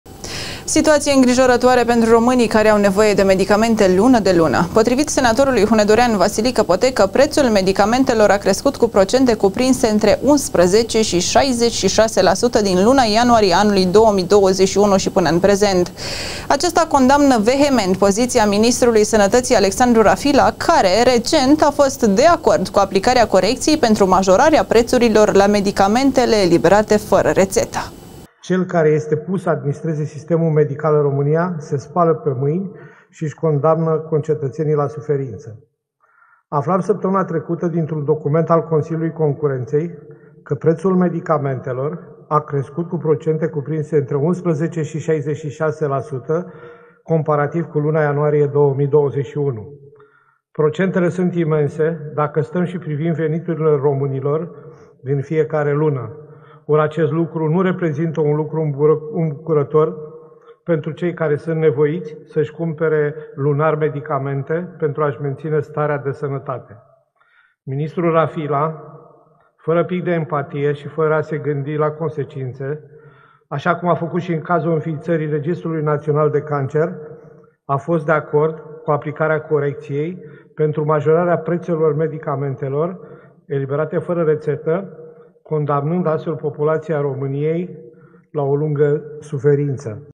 V. POTECĂ: MINISTRUL SĂNĂTĂȚII ÎȘI CONDAMNĂ CONCETĂȚENII LA SUFERINȚĂ
„PREȚUL MEDICAMENTELOR A CRESCUT CU 11-66% FAȚĂ DE ANUL 2021, (...) PROCENTELE SUNT IMENSE”
        Situație îngrijorătoare pentru românii care au nevoie de medicamente lună de lună. Potrivit senatorului hunedorean Vasilică Potecă, prețul medicamentelor a crescut cu procente cuprinse între 11 și 66%, din luna ianuarie a anului 2021 și până în prezent. Acesta condamnă vehement poziția ministrului Sănătății, Alexandru Rafila, care, recent, a fost de acord cu aplicarea corecției pentru majorarea prețurilor la medicamentele eliberate fără rețetă.